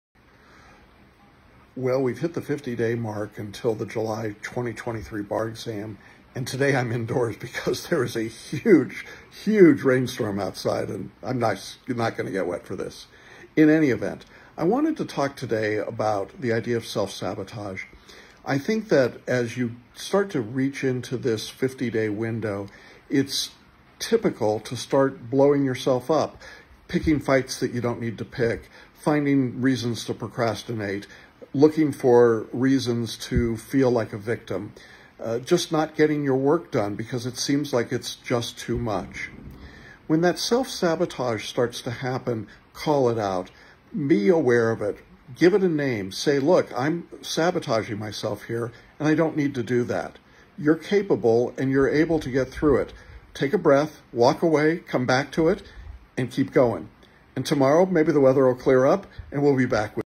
We’re at the 50 day mark in our countdown to the July2023BarExam
We’re at the 50 day mark in our countdown to the July2023BarExam. Today our topic is self sabotage.